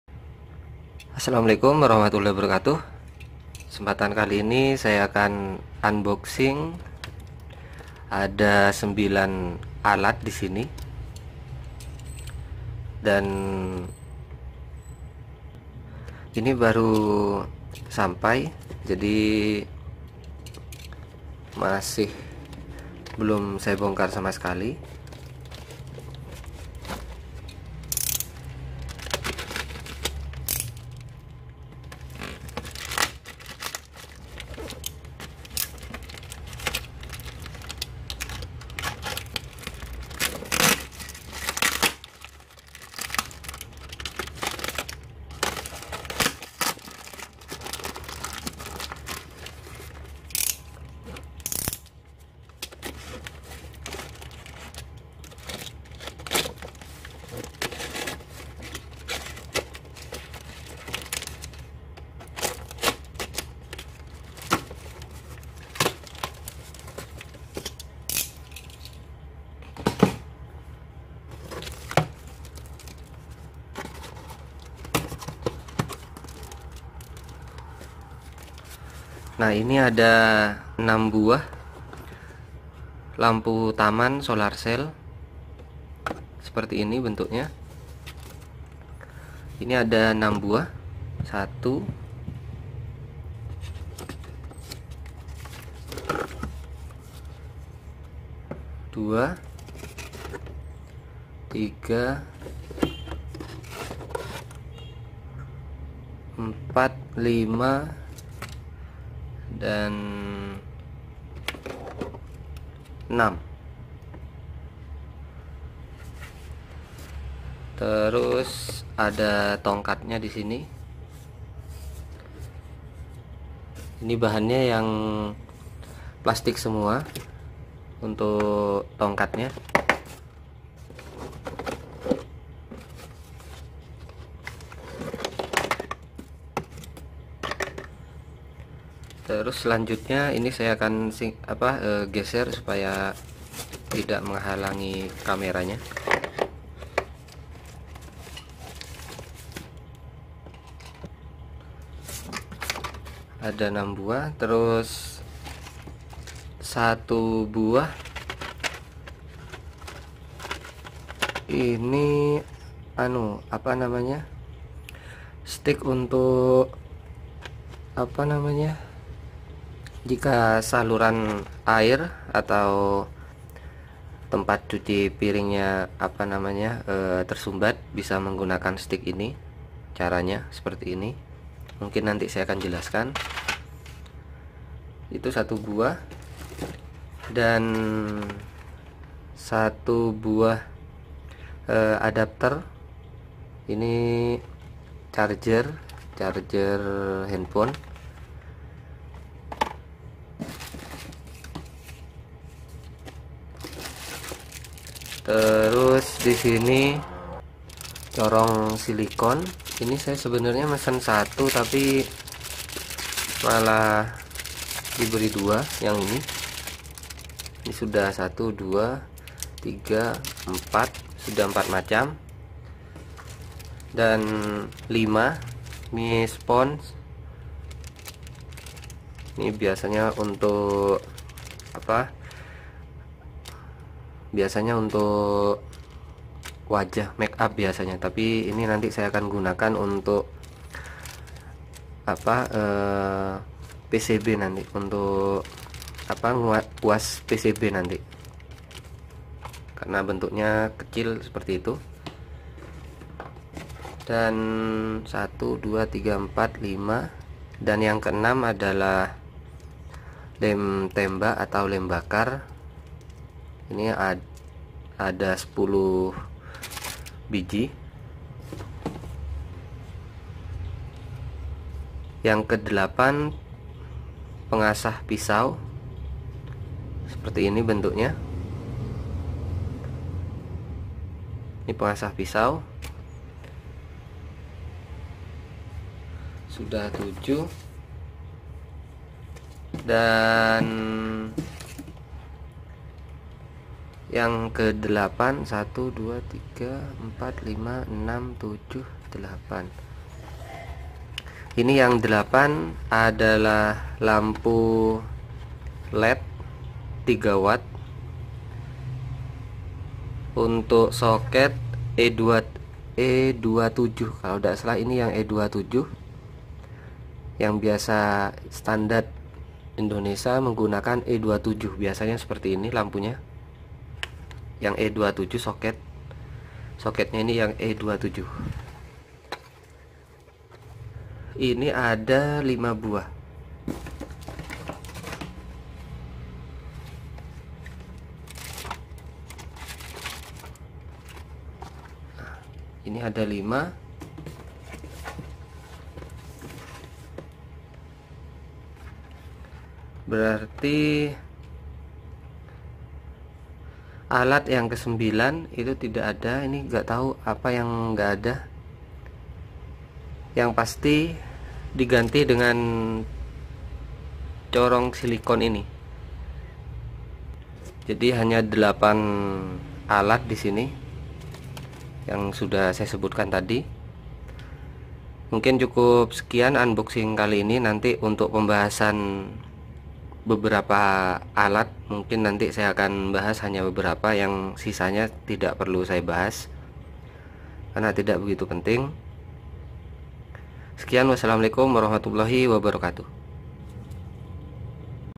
Unboxing 9 Macam Alat Unik Bermanfaat Hilang 1 :D
- lampu taman solar cell mini
- corong lipat silikon
- charger hp
- pengasah pisau
- lampu led 220volt soket e27
- spon rias mini
- lem bakar/tembak
- pembersih saluran air